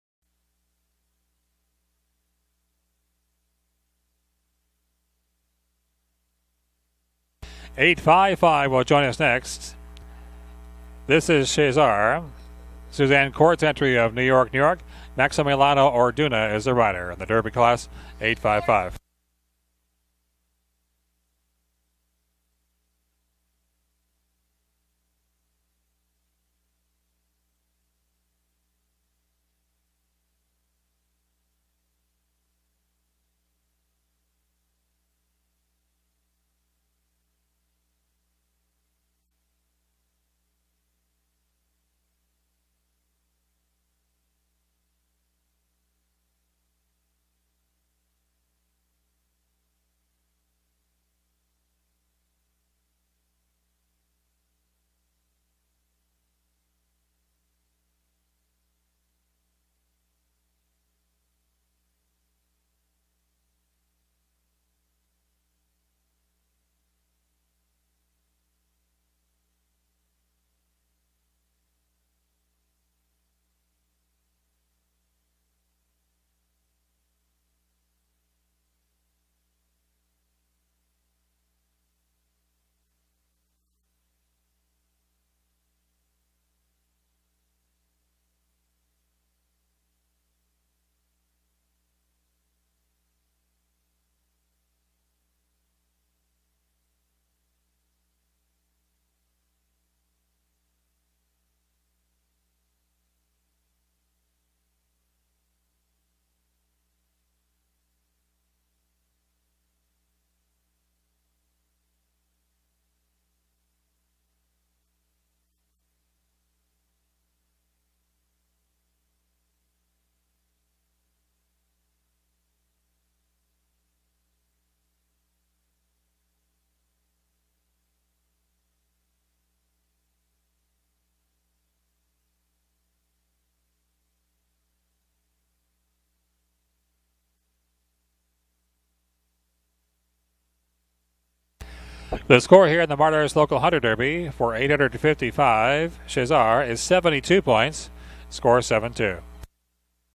Video of CHESAR ridden by MAXIMILIANO ORDUNA from ShowNet!
CHESAR ridden by MAXIMILIANO ORDUNA at 2018 Hampton Classic Local Day.  This round is from class 420, $10,000 MARDERS LOCAL HUNTER DERBY and finished 21 of 39.